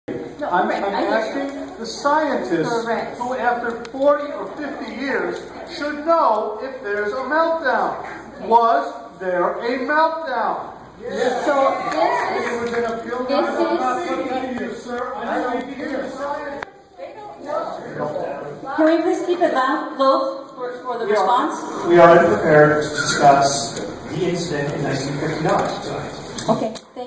Santa Susana Town Hall - "Was There a Meltdown?"
When a member of the audience asked point blank if a partial meltdown had ever happened at the site, DTSC Santa Susana Project Director Ray LeClerc said he was "unprepared" to talk about it and the department would post something about the "incident" on the DTSC website soon. Now, the DTSC can't even admit a partial nuclear meltdown that is a documented part of history.